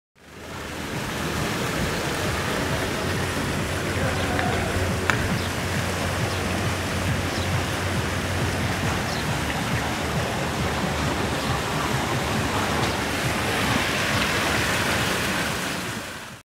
Hurghada Seagull Beach Resort
Theres nothing like an early morning stroll through the wonderful grounds of SeagullBeachResort and listening to the water cascading in the ornate fountains...